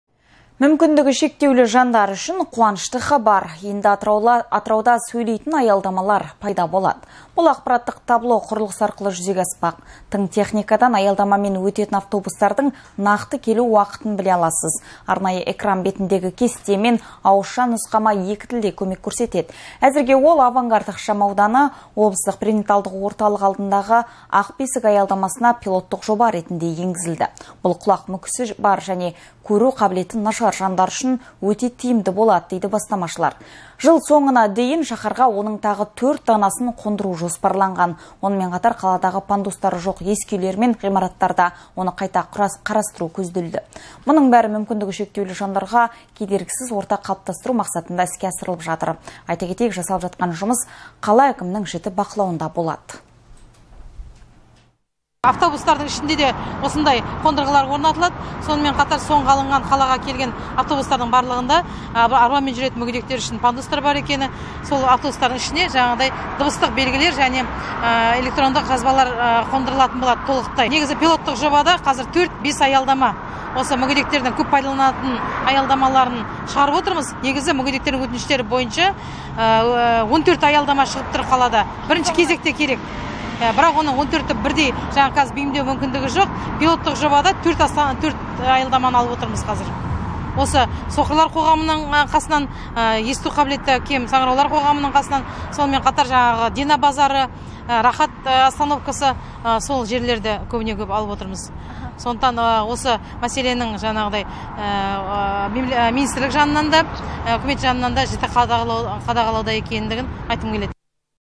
АЯЛДАМАЛАР "СӨЙЛЕЙДІ"
Мүмкіндігі шектеулі жандар үшін қуанышты хабар! Енді Атырауда сөйлейтін аялдамалар пайда болады. Бұл "ақпараттық табло" құрылғысы арқылы жүзеге аспақ. Тың техникадан аялдамамен өтетін автобустардың нақты келу уақытын біле аласыз. Арнайы экран бетіндегі кесте мен ауызша нұсқама екі тілде көмек көрсетеді.  Әзірге ол авангард ықшам ауданы, "Облыстық перинаталдық орталық" алдындағы "Ақ бесік" аялдамасына пилоттық жоба ретінде енгізілді. Бұл құлақ мүкісі бар және көру қабілеті нашар жандар үшін өте тиімді болады дейді бастамашылар. Жыл соңына дейін шаhарға оның тағы 4 данасын қондыру жоспарланған. Онымен қатар, қаладағы пандустары жоқ ескі үйлер мен ғимараттарда оны қайта қарастыру көзделді. Мұның бәрі мүмкіндігі шектеулі жандарға кедергісіз орта қалыптастыру мақсатында іске асырылып жатыр. Айта кетейік, жасалып жатқан жұмыс қала әкімінің жіті бақылауында болады.